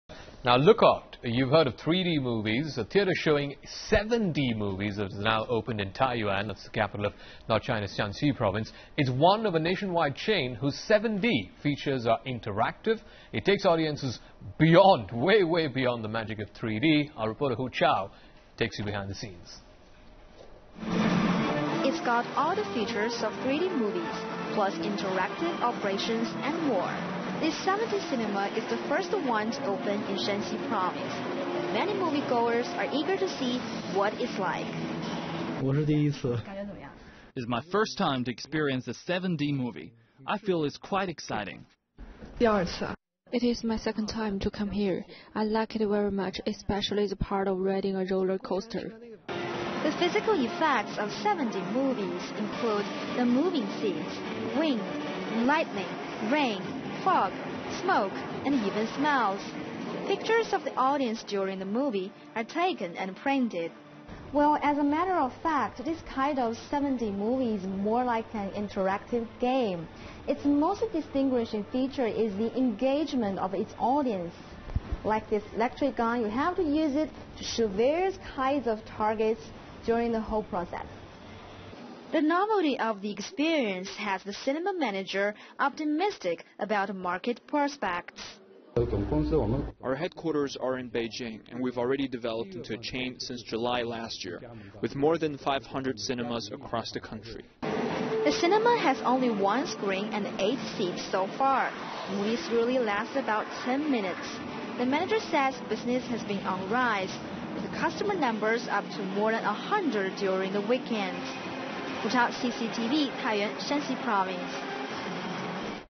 7D CINEMA OPENS IN NORTH CHINA
A theater showing 7D movies has opened in Taiyuan, the capital of North China's Shanxi Province. It's one of a nation-wide chain, whose 7D features are interactive, taking audiences beyond the magic of 3D.